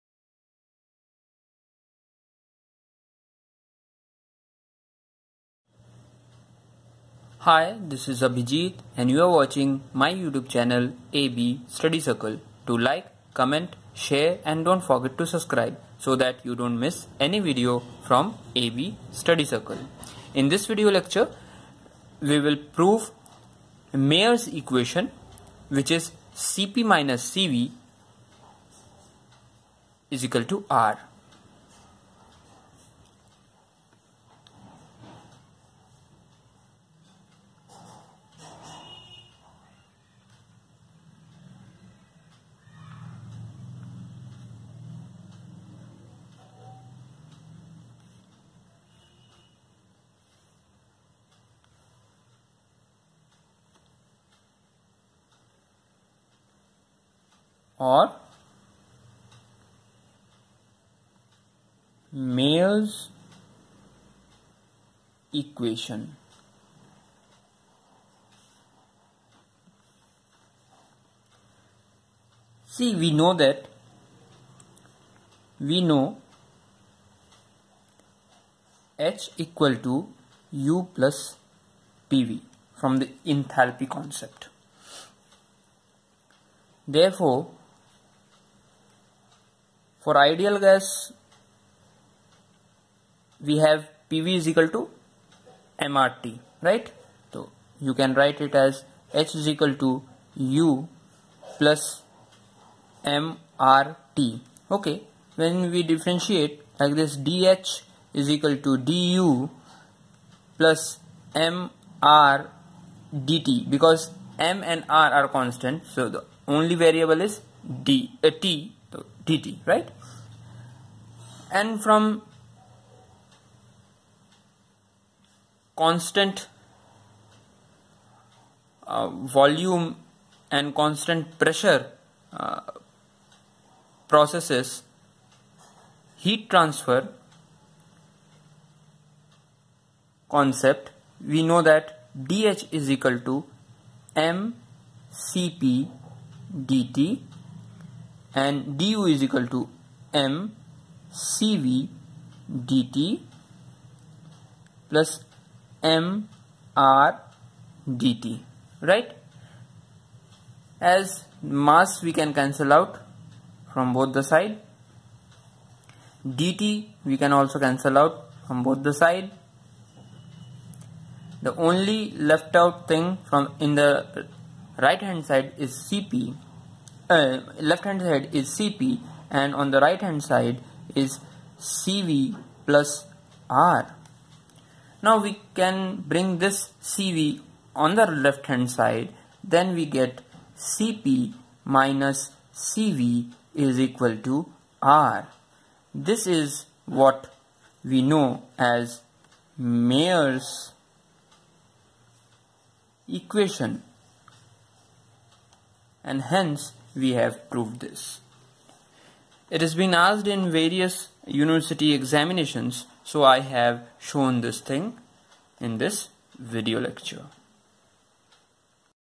Proof | Meyer's Equation | Cp-Cv=R
Meyer's Equation is a very important equation as it relates specific heat for constant volume and pressure process with gas constant.